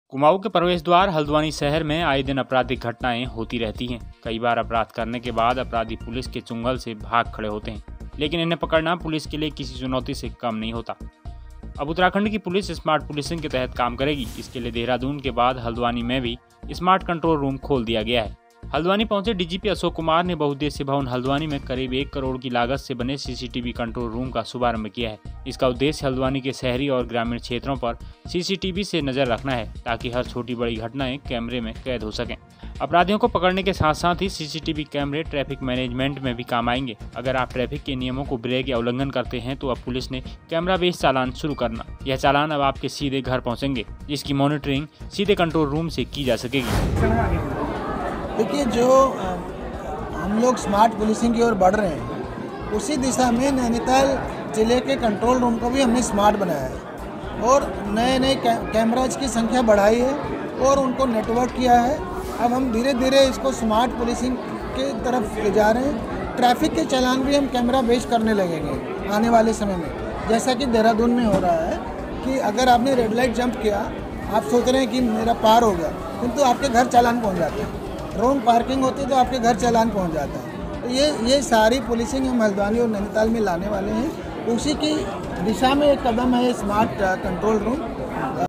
हल्द्वानी में खुला पुलिस कंट्रोल रुम| Smart Policing Uttrakhand | Nainital Police
कुमाऊं के प्रवेश द्वार हल्द्वानी शहर में आए दिन अपराधिक घटनाएं होती रहती हैं, कई बार अपराध करने के बाद अपराधी पुलिस के चुंगल से भाग खड़े होते हैं, लेकिन इन्हें पकड़ना पुलिस के लिए किसी चुनौती से कम नहीं होता।

अब उत्तराखंड की पुलिस स्मार्ट पुलिसिंग के तहत काम करेगी, इसके लिए देहरादून के बाद हल्द्वानी में भी स्मार्ट कंट्रोल रूम खोल दिया गया है,

हल्द्वानी पहुंचे डीजीपी अशोक कुमार ने बहुउद्देशीय भवन हल्द्वानी में करीब एक करोड़ रुपये की लागत से बने सीसीटीवी कंट्रोल रूम का शुभारंभ किया इसका उद्देश्य हल्द्वानी के शहरी और ग्रामीण क्षेत्रों पर सीसीटीवी से नजर रखना है, ताकि हर छोटी बड़ी घटनाएं कैमरे में कैद हो सकें।

अपराधियों को पकड़ने के साथ-साथ ही सीसीटीवी कैमरे ट्रैफिक मैनेजमेंट में भी काम आएंगे, अगर आप ट्रैफिक के नियमों को ब्रेक करते हैं तो अब पुलिस ने कैमरा बेस चालान करना शुरू कर दिया है। यह चालान अब सीधे आपके घर पहुंचेगा, अब इन सब की मॉनिटरिंग कंट्रोल रूम से हो सकेगी